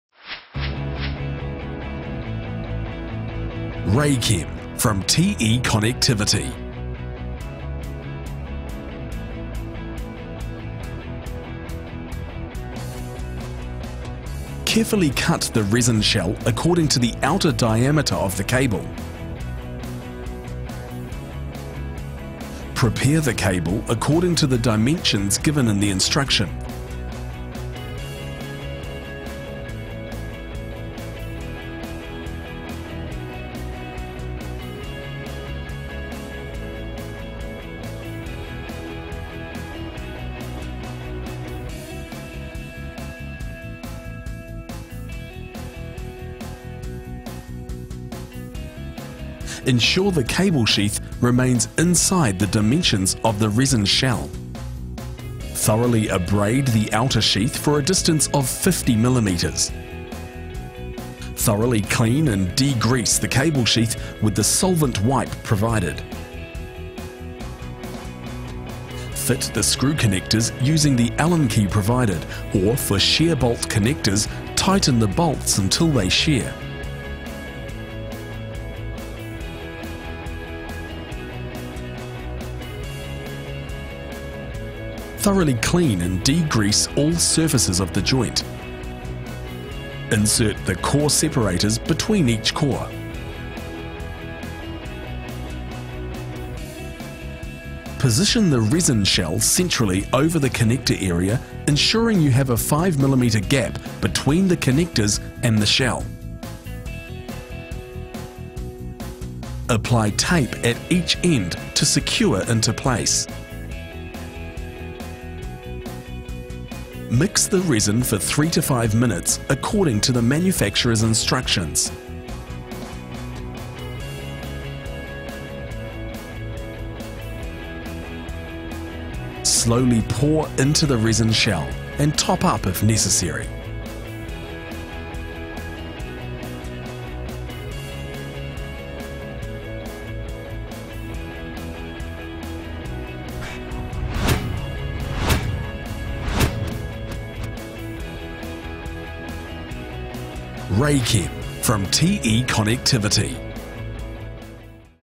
Raychem LV multicore resin joint
This video demonstrates how to correctly install a Raychem LV multicore resin joint, from manufacturers TE Connectivity.

All Round Supplies stocks an extensive range of jointing and termination solutions, take a look